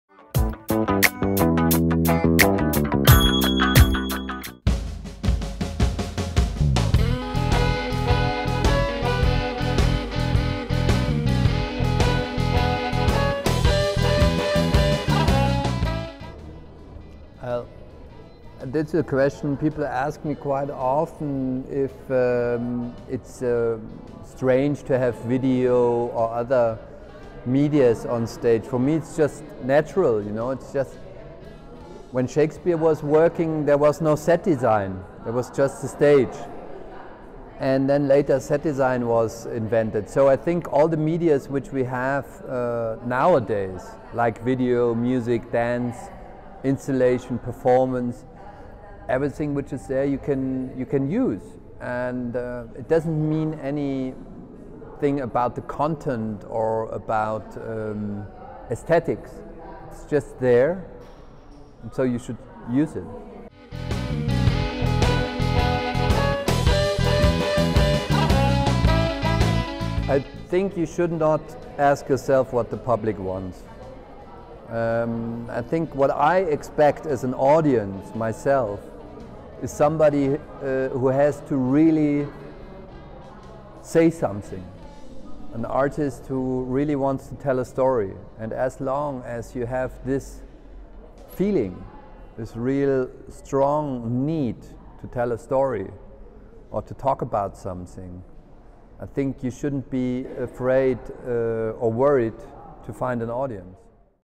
Thomas Ostermeier no Congresso CULT 2012
Diretor geral do teatro Schaubühne, em Berlim, na Alemanha, na entrevista a seguir Thomas Ostermeier dá seu parecer sobre a presença do cinema e da música nas peças teatrais e sobre a relação entre o desejo do público e o desejo do artista. Ele veio ao Brasil para participar da mesa "Teatro: Os Novos Repertórios" do 4° Congresso Internacional CULT de Jornalismo Cultural.